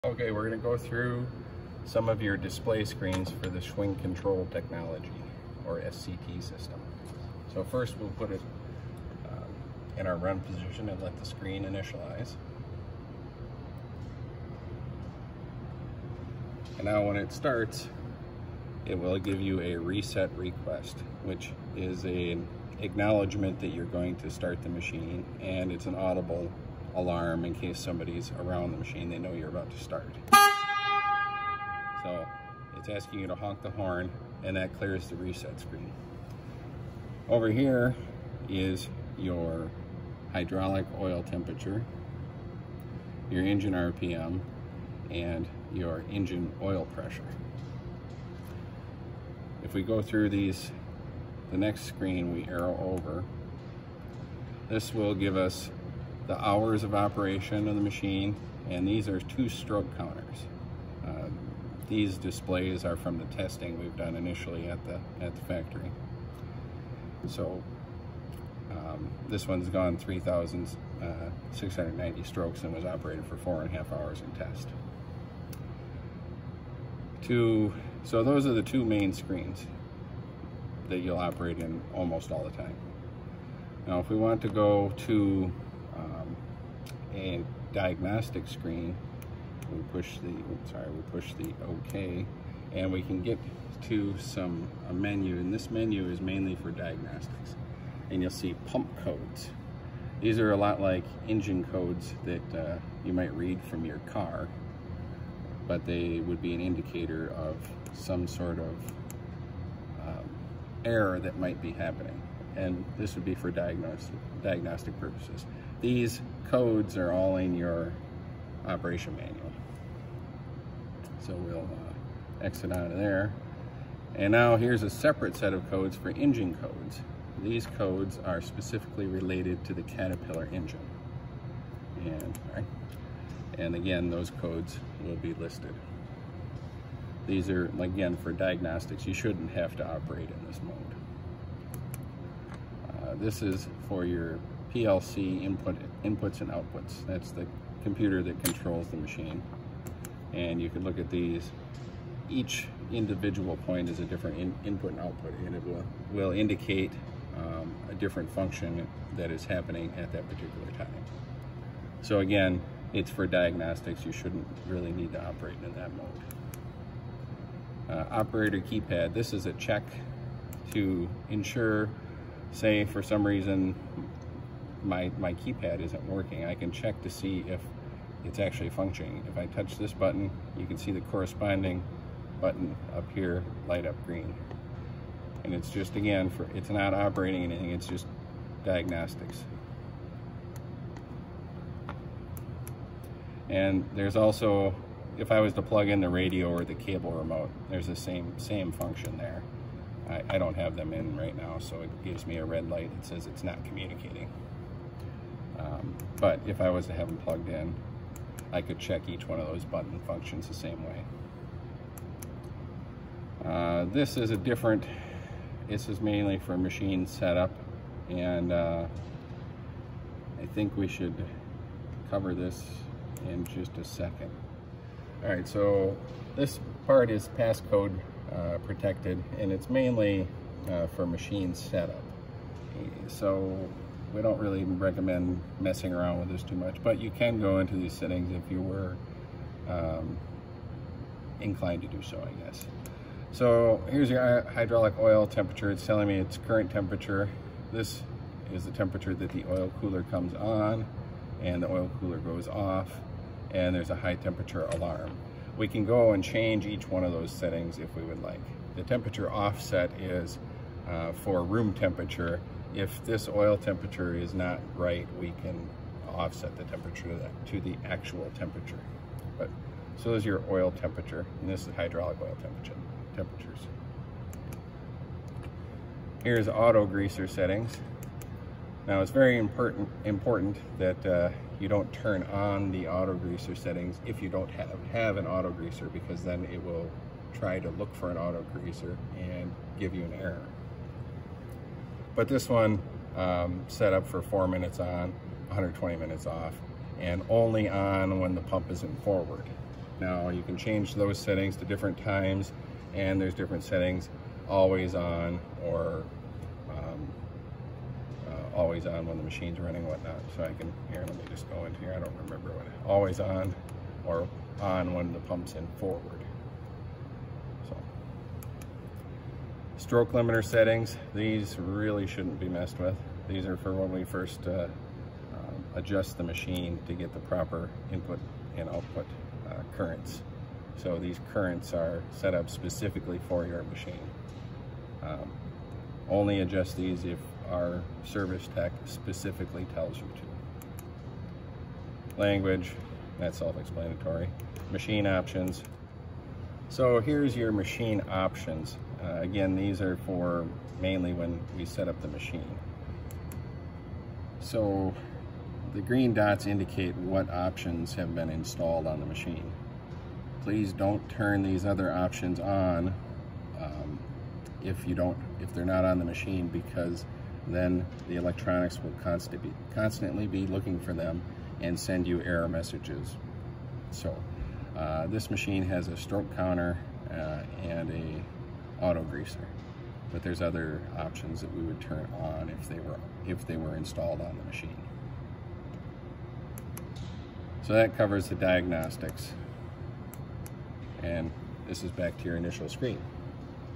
Display Overview Of The SCT System
SCHWING: SCT System | Showcase
Overview of the SCT controller and display screen for SCHWING SP concrete pumps.

0:00 Introduction
0:37 Main Screen
1:20 Diagnostic Screens
2:47 Operator Keypad
3:33 Machine Set-up
5:19 Stroke Limiter Settings
5:46 Machine Options

Schwing Inc.